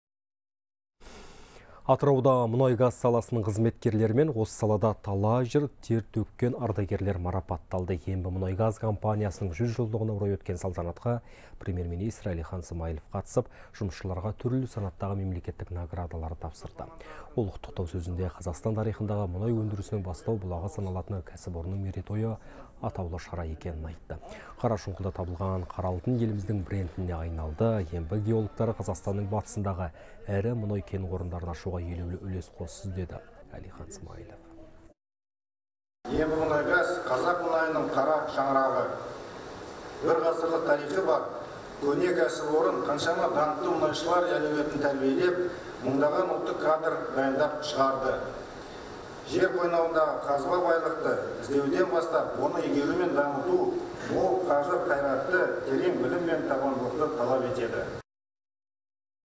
Атырауда мұнай-газ саласының қызметкерлері марапатталды
Атырауда мұнай-газ саласының қызметкерлері мен осы салада талай жыл тер төккен ардагерлер марапатталды.
«Ембімұнайгаз» компаниясының 100 жылдығына орай өткен салтанатқа Премьер-министр Әлихан Смайылов қатысып, жұмысшыларға түрлі санаттағы мемлекеттік наградаларды тапсырды.